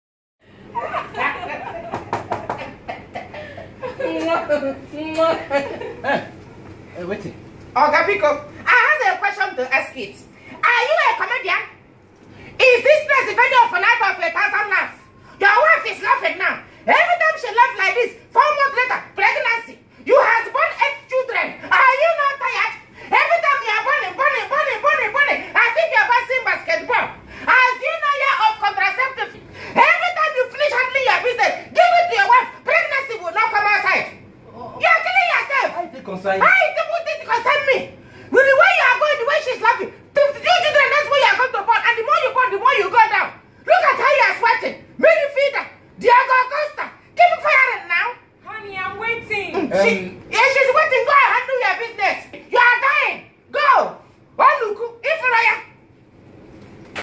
Nkechi (Honey & Banana)
Don't wait till you have 10 kids before you start a contraceptive plan. With this package you still fit enjoy your spouse wella.

Listen to Nkechi :-D

cc @nedu_wazobiafm